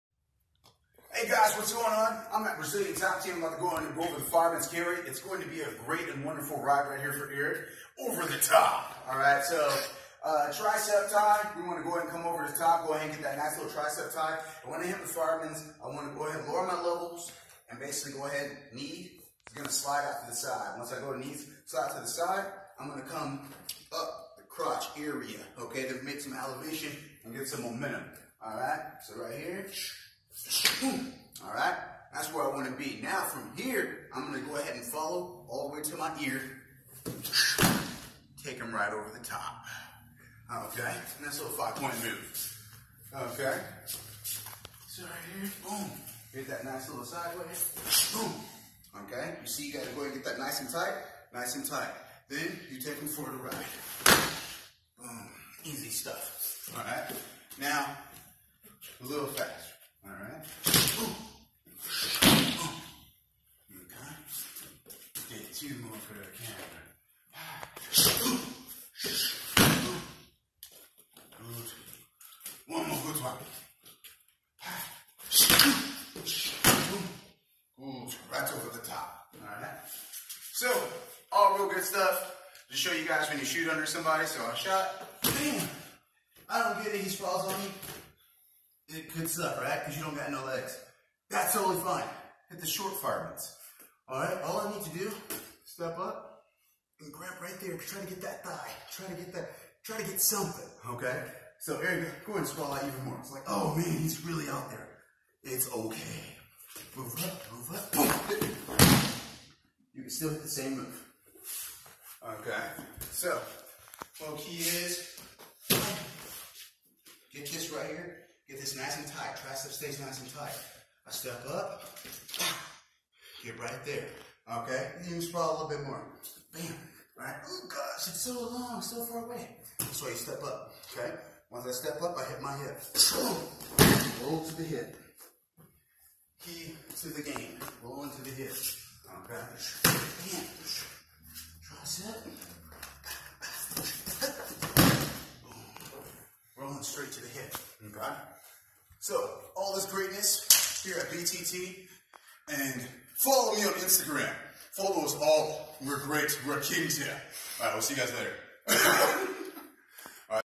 The Wrestling Library: Fireman's Carry & Short Fireman's 👨‍🚒🚒
The Fireman's Carry is an amazing Takedown and an amazing way to take your opponent straight to his back quickly! Here's a quick overview!